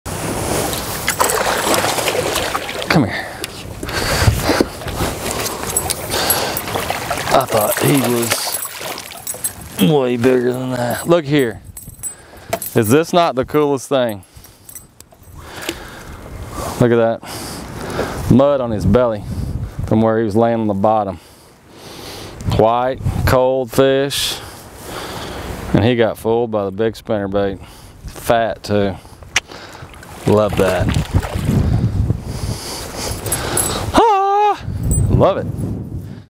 LOOK for THIS! Attention to the Details Can Change Your Day!
LOOK for THIS! Attention to the Details Can Change Your Day! – When Jason Christie is throwing a spinnerbait in the winter/early prespawn, he pays attention to details like THIS!

Videography & Edit by Creating Outdoors